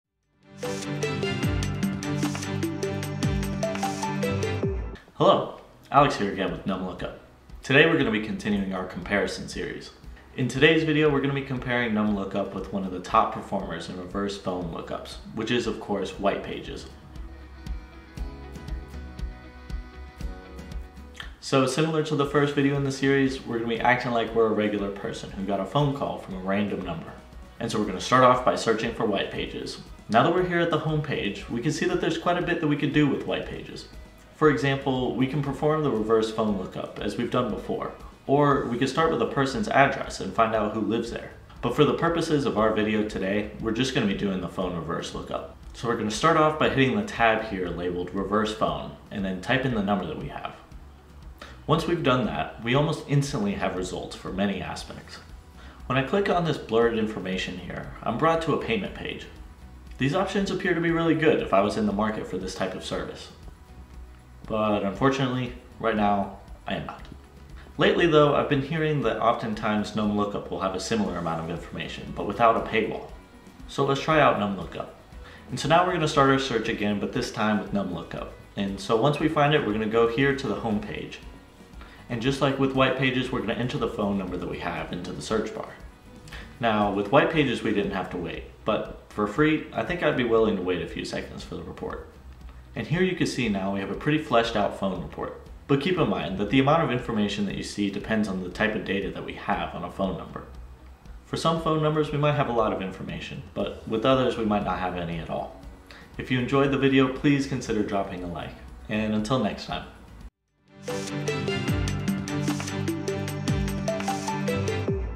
NumLookup vs WhitePages - Which One is Better?
Join Alex as he dives deep into the world of reverse phone lookup services! 📱 In this video, we'll be pitting NumLookup against Whitepages, two giants in the game. If you've ever needed to figure out who's behind that mysterious phone number or wanted to verify a potential caller's identity, this is the video for you.